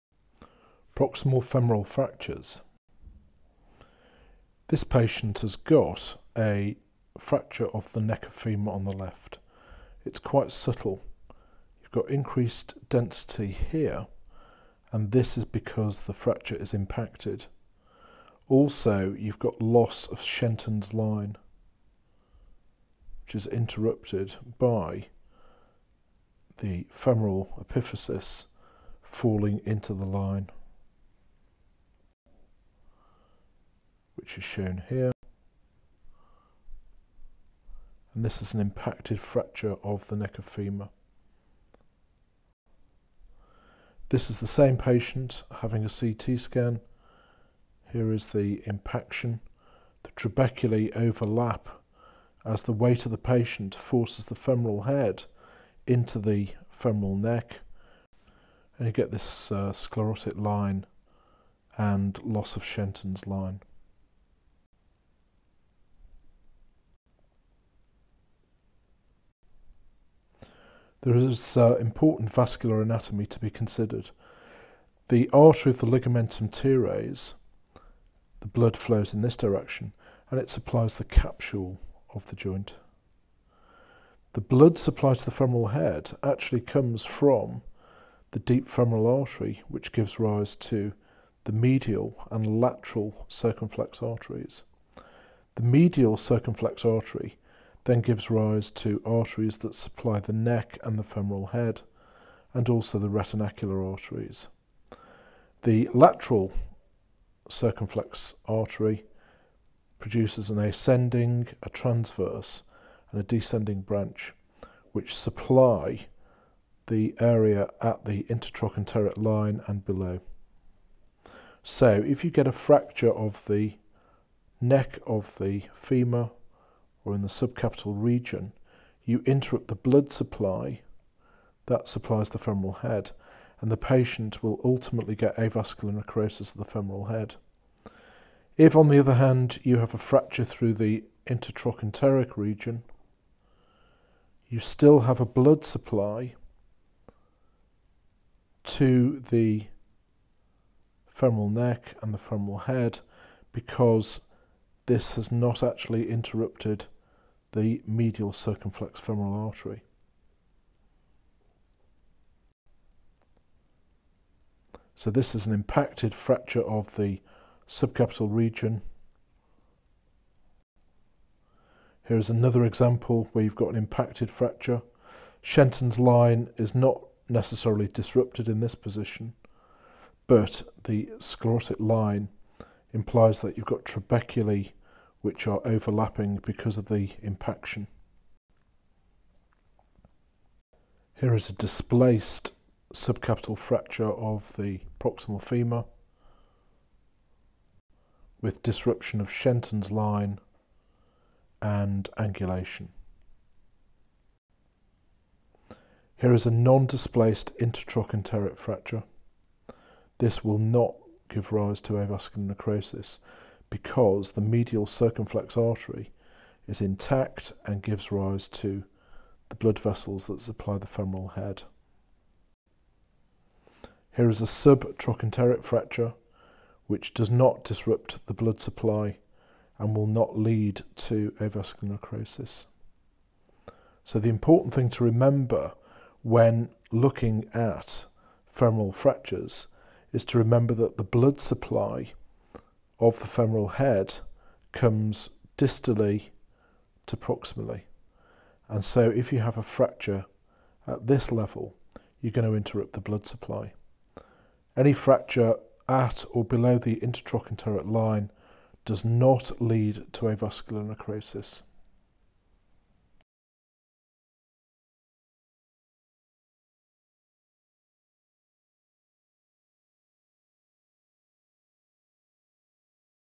Know your hip fractures and the importance of arterial anatomy of the proximal femur
This video emphasises the importance of understanding the arterial anatomy of the femoral neck and head in determining which fracture is likely to lead to avascular necrosis of the femoral head